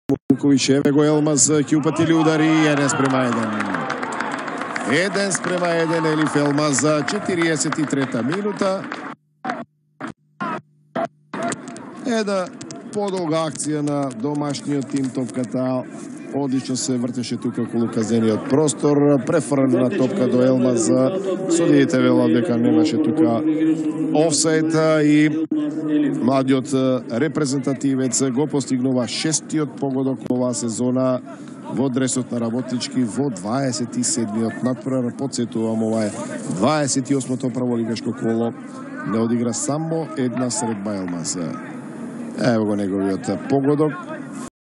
Работнички - Македонија ЃП 1-1 (гол на Елиф Елмас)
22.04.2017 - ПМФЛ-28. коло Работнички - Македонија ЃП 1-1 (гол на Елиф Елмас)